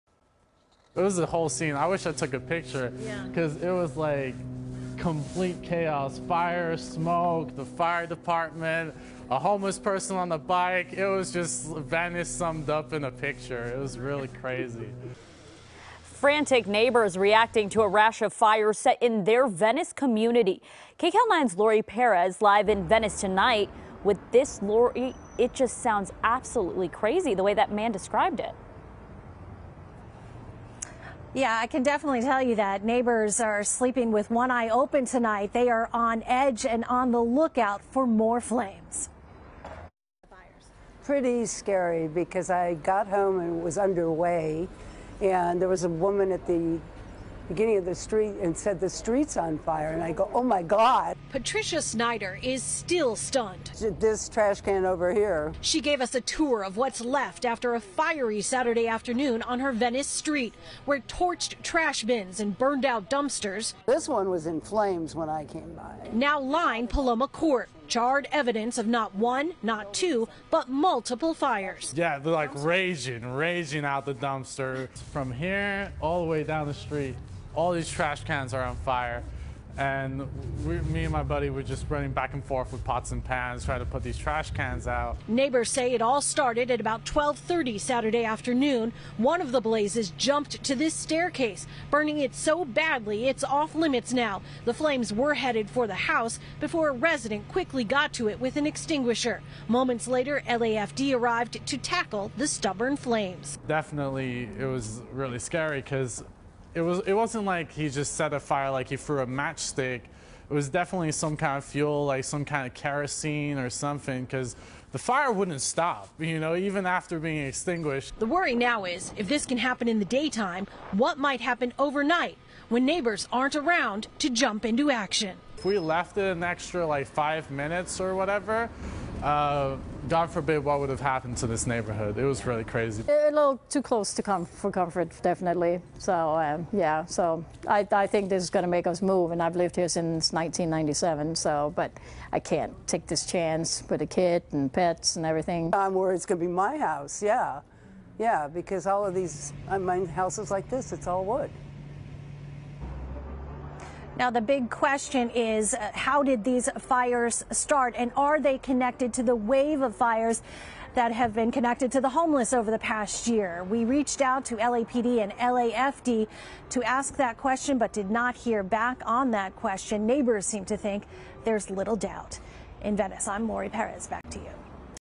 String of Fires Has Venice Residents Worried
After a string of fires in recent weeks, including one trash fire that nearly took out several homes, Venice residents are calling for a change from city officials before things get out of hand.